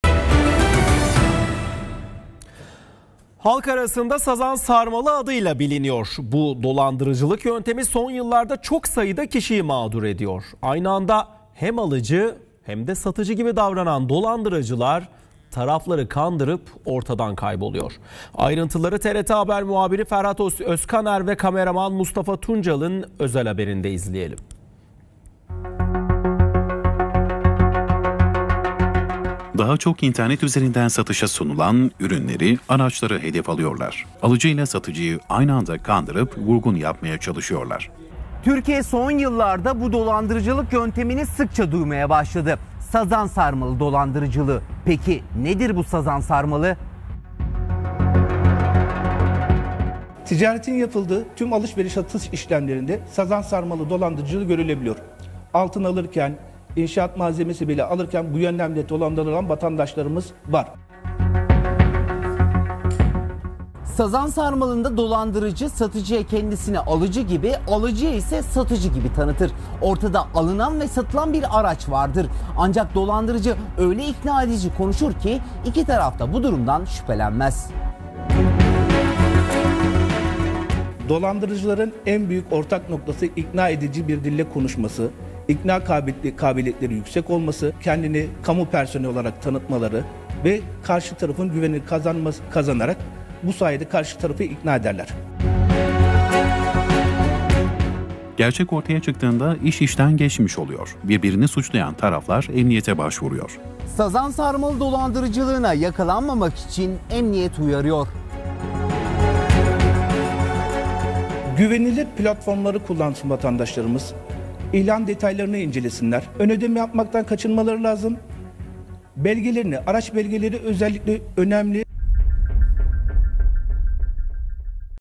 SAZAN SARMALI DOLANDIRICILIĞI!
Türkiye son yıllarda bu dolandırıcılık yöntemini sıkça duymaya başladı, ‘’Sazan sarmalı dolandırıcılığı’’ peki nedir bu sazan sarmalı?